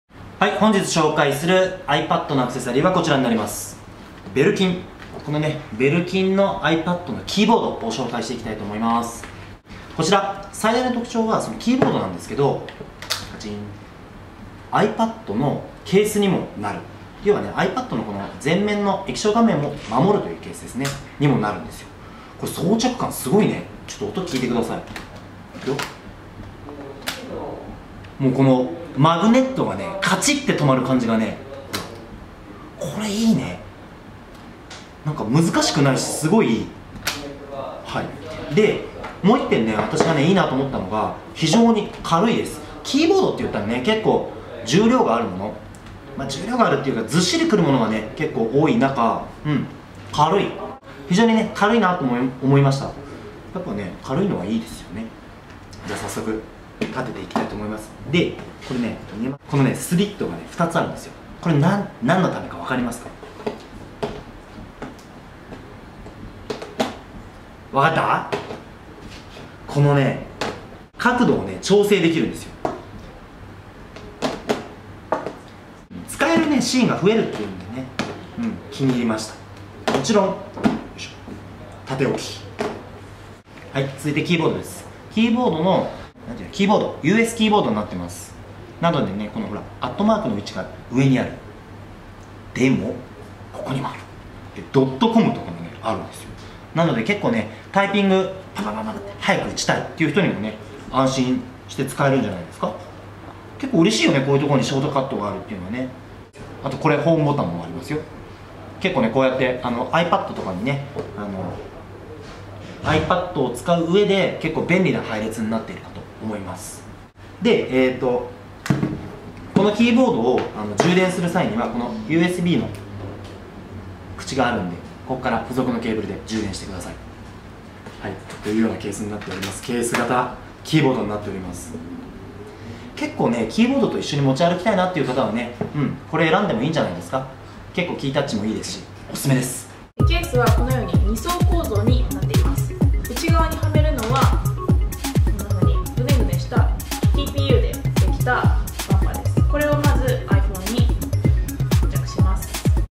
iPadがノートパソコンに変身するキーボードカバー - iPad アクセサリ
MacBook AirみたいにiPadが変身するカバー兼キーボードです。軽くて薄いので、持ち運びにも便利です。

▼商品レビューで詳しく見たい方はこちら

出演：kazuend ◆ たのしいiPhoneグッズ紹介！ 『バイヤーチャンネル24』に登録する ↓↓